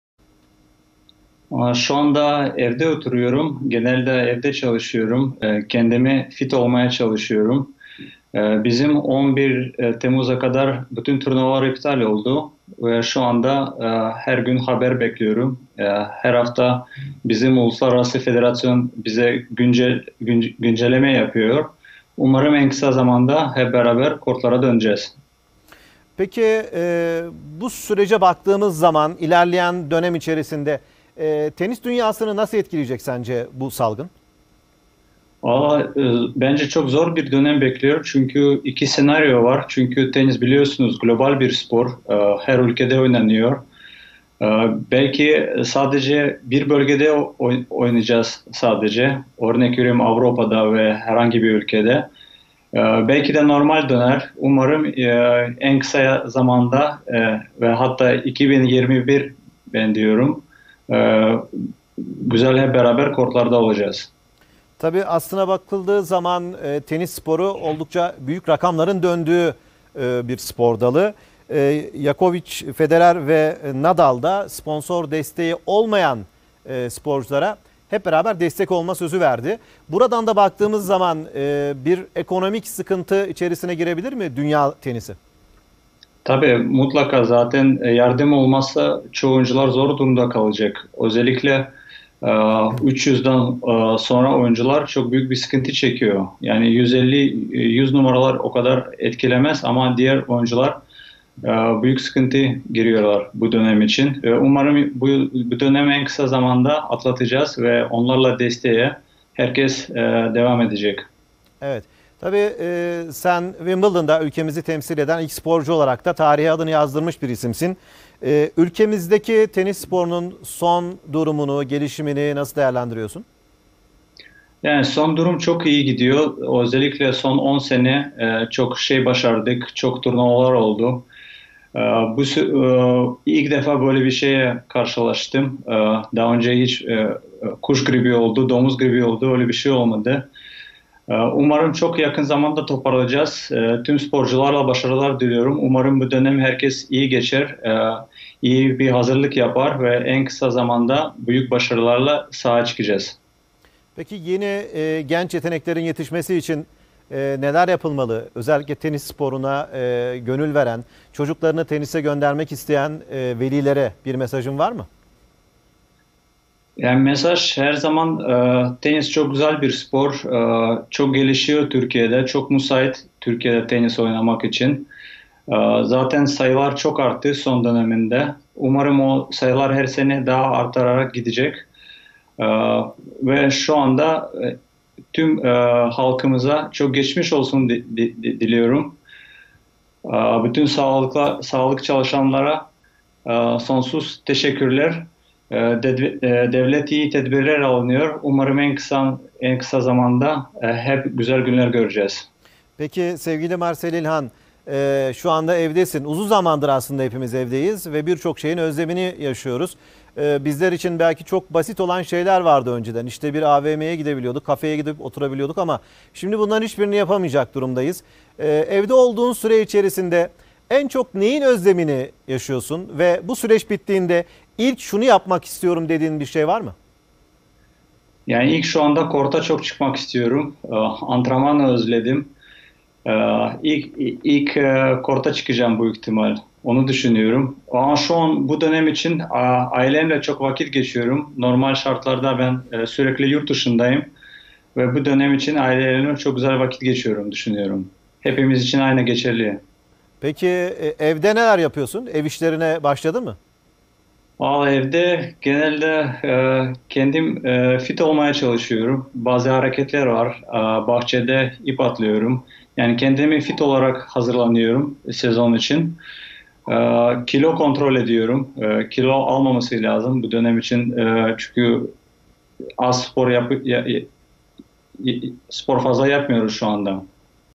"Tenisi zor bir dönem bekliyor"
Marsel Ilhan, TRT Spor'da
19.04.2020